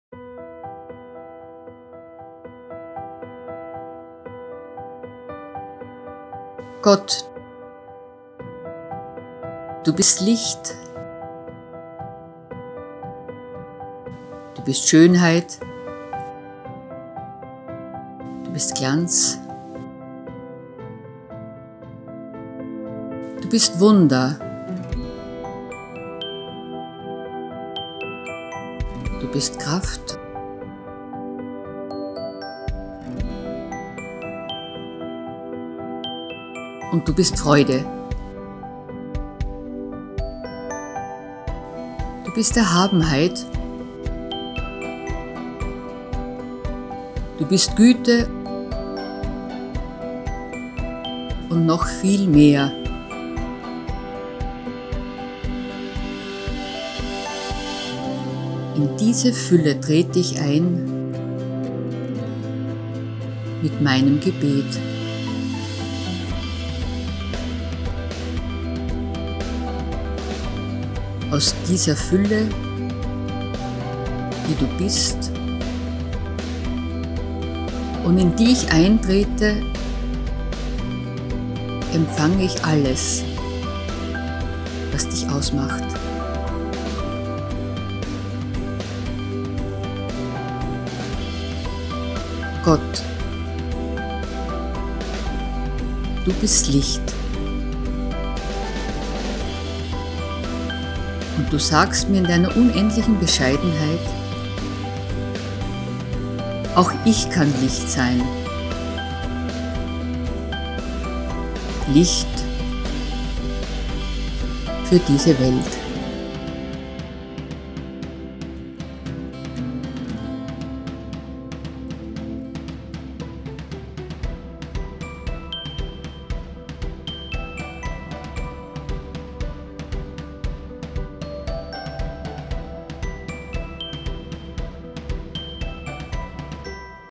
Meditation in der Fastenzeit: Dienstag, 23.März
Für jeden Tag der Fastenzeit (die Sonntage sind ausgenommen) erscheint hier ein Video mit einer kurzen Meditation.
Sprecherin: Annelies Tengler
Text: Josef Hackl